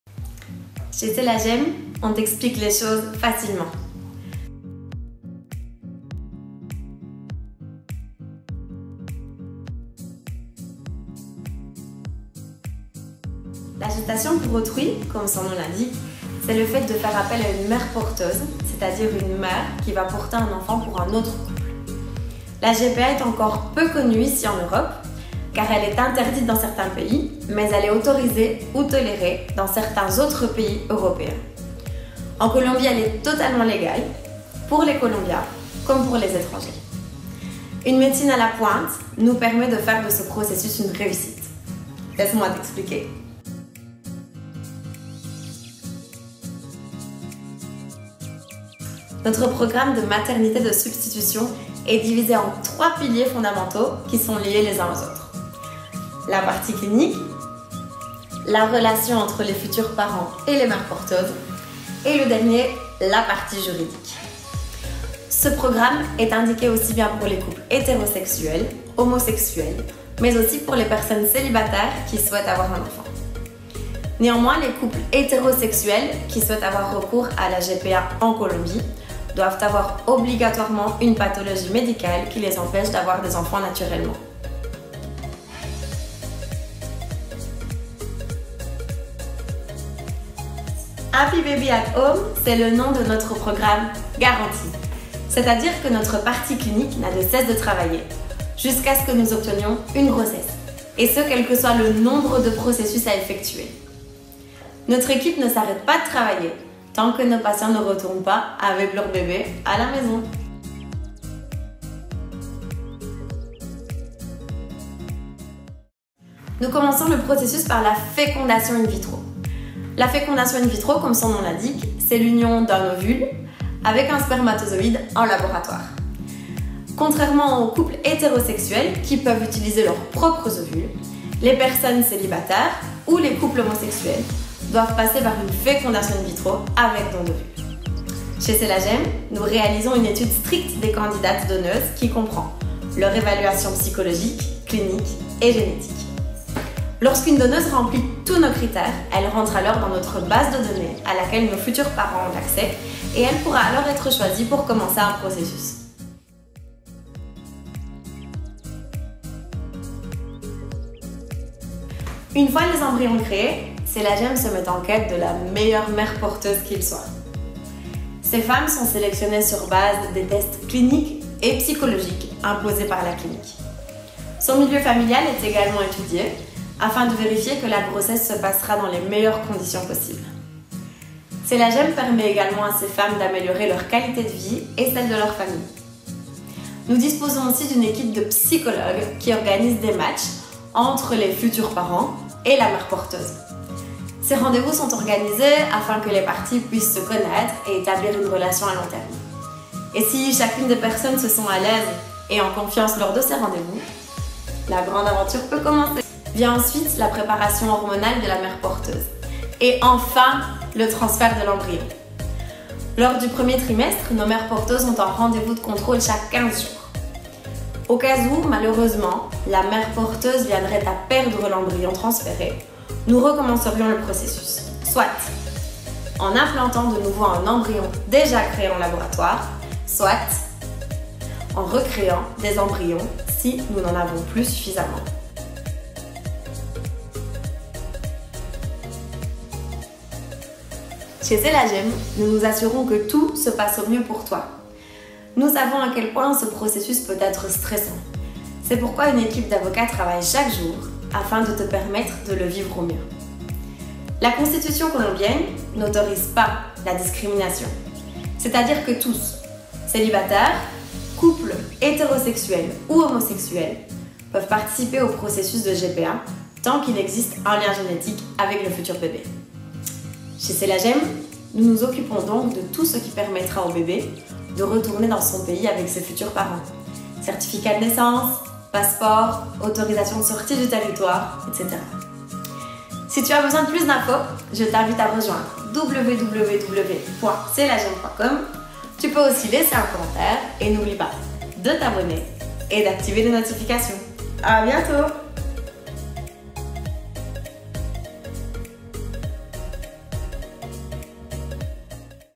Gestation pour autrui en Colombie
Qu'est-ce que la maternité de substitution?
La gestation pour autrui, comme son nom l'indique, est le fait d'appeler une mère porteuse, une mère qui portera un enfant pour un autre couple.
La maternité de substitution est encore peu connue ici en Europe, interdite dans certains pays mais autorisée ou tolérée dans certains autres pays européens.
En Colombie, c'est tout à fait légal pour les Colombiens et les étrangers.
La médecine de pointe nous permet de faire de ce processus un succès, je m'explique.